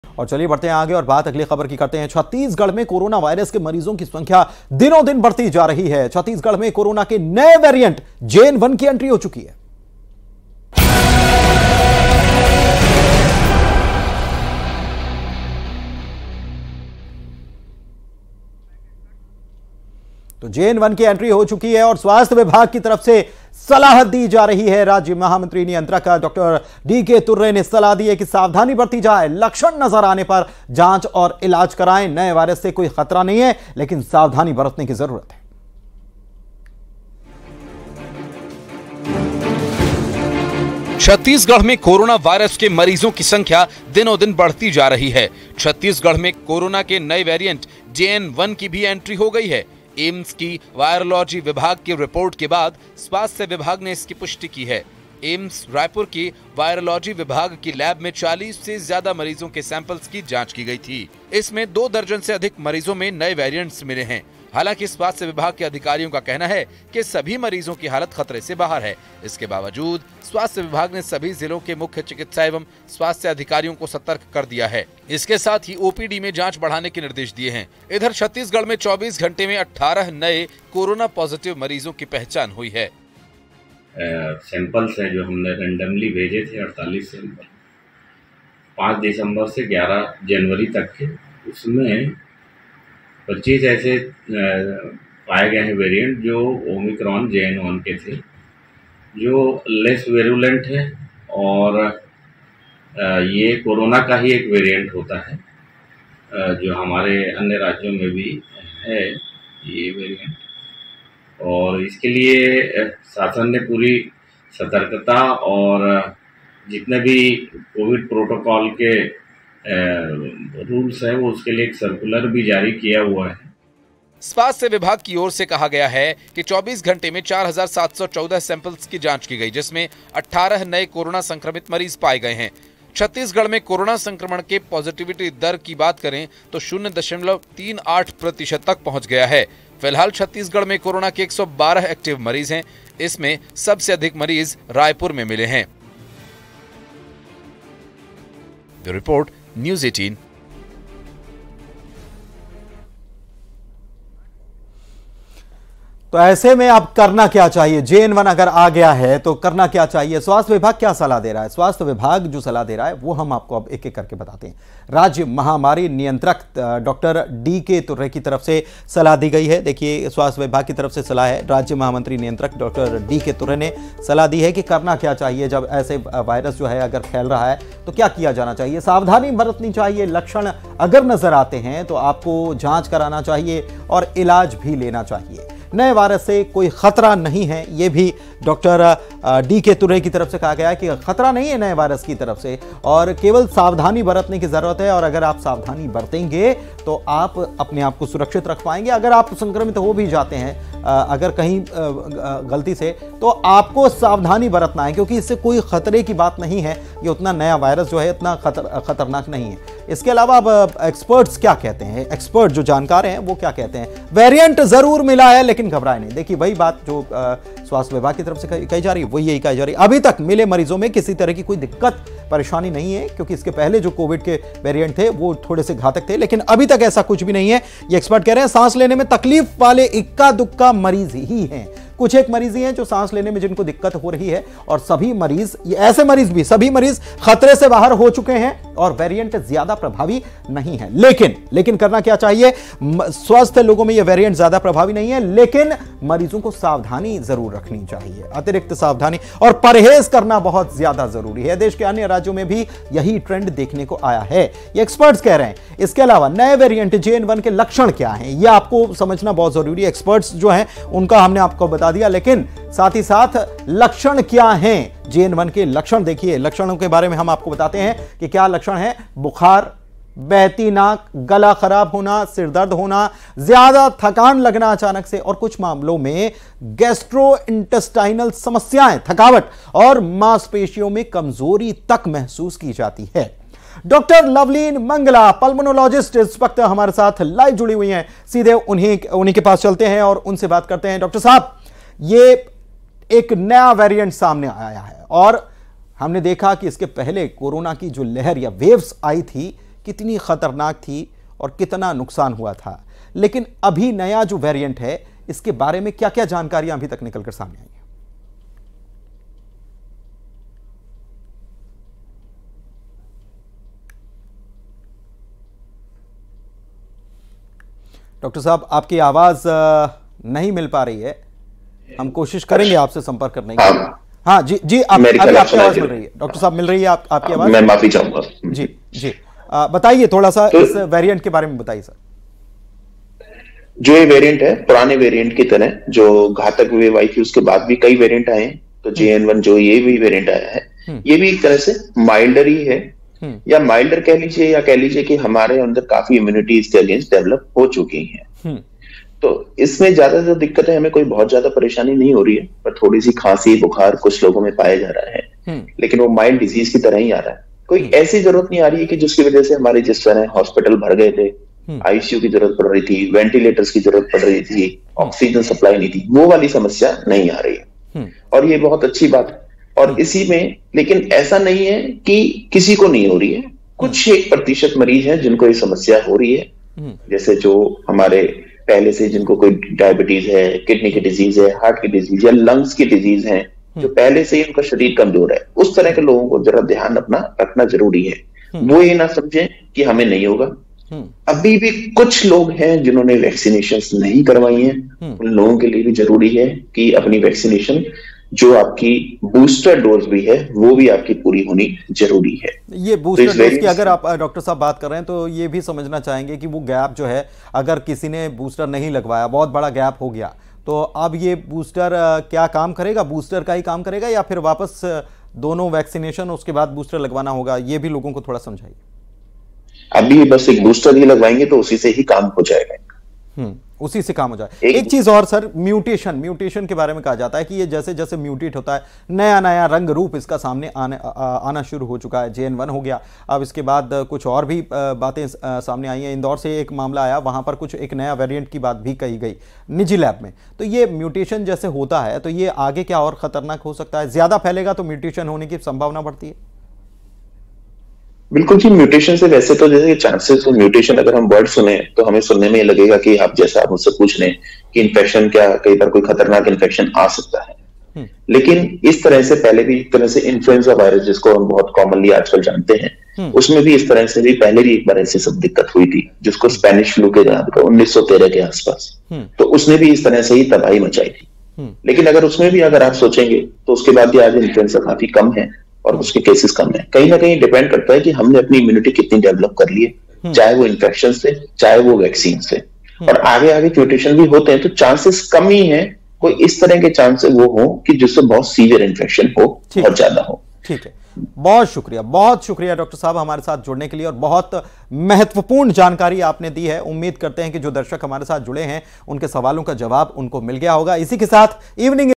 Coronavirus Cases in Chhattisgarh: छत्तीसगढ़ में मरीजों की बढ़ी संख्या, JN-1 की हुई Entry | Top News
छत्तीसगढ़ में मरीजों की बढ़ी संख्या, JN-1 की हुई Entry